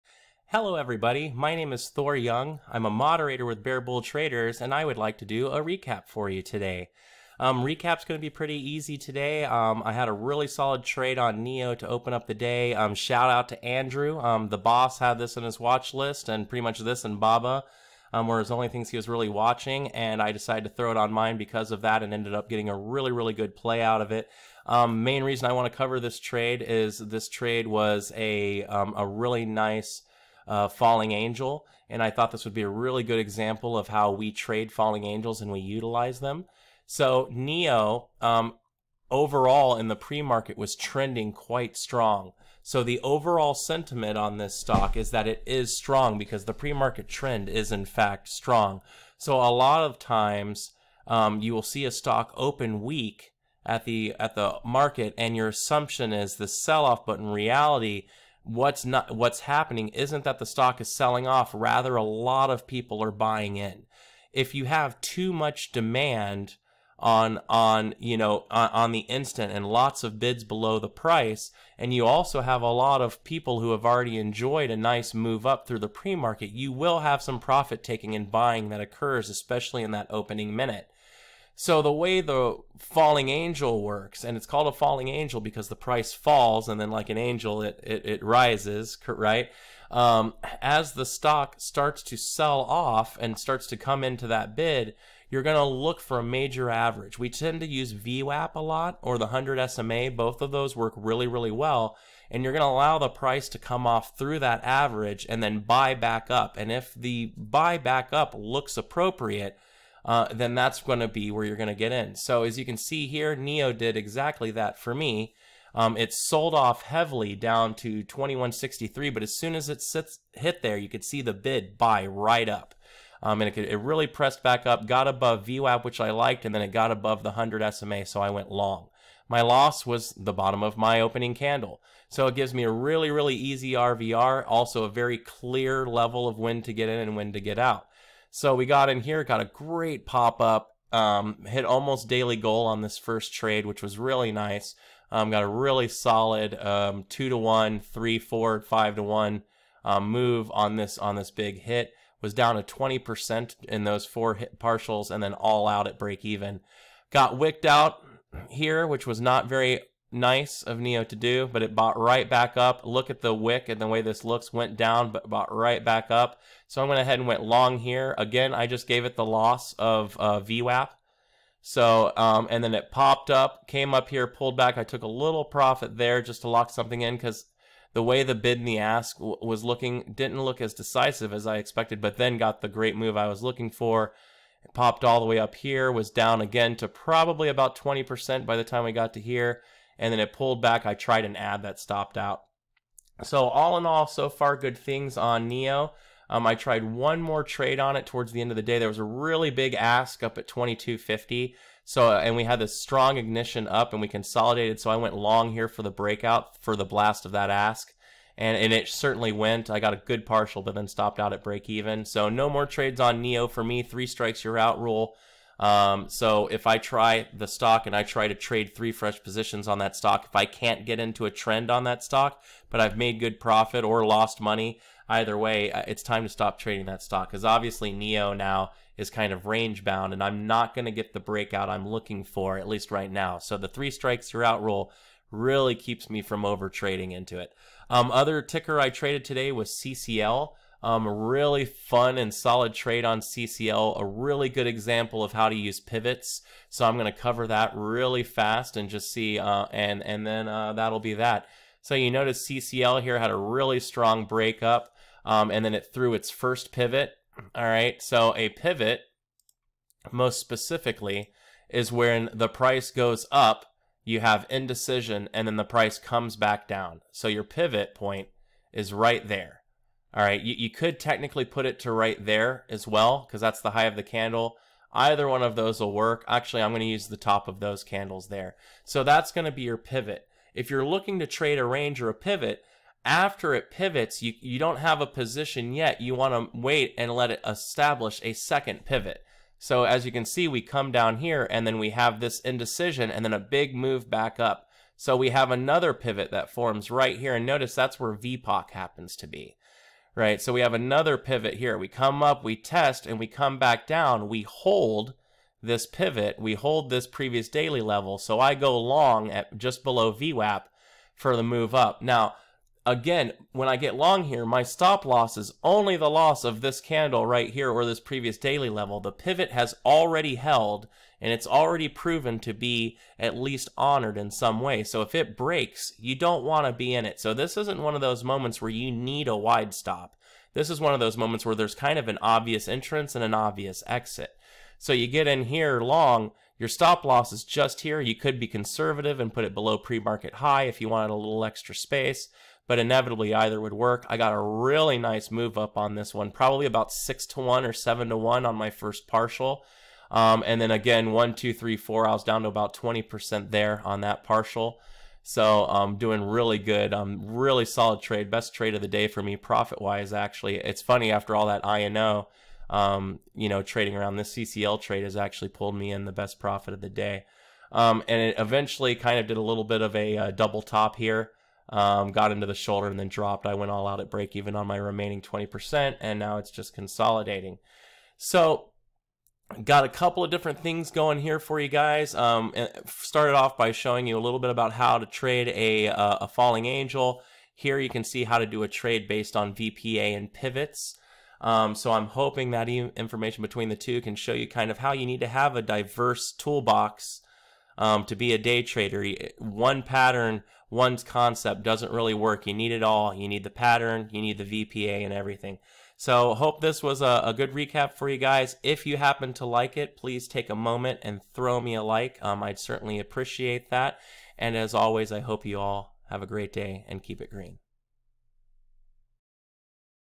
Thor's Day Trading Recap - 093020 Trades using VPA, Falling Angel, and Pivots.
🔥 Check Bear Bull Traders Books!
👉"How to Day Trade for a Living”
👉"Advanced Techniques in Day Trading”
👉”How to Swing Trade”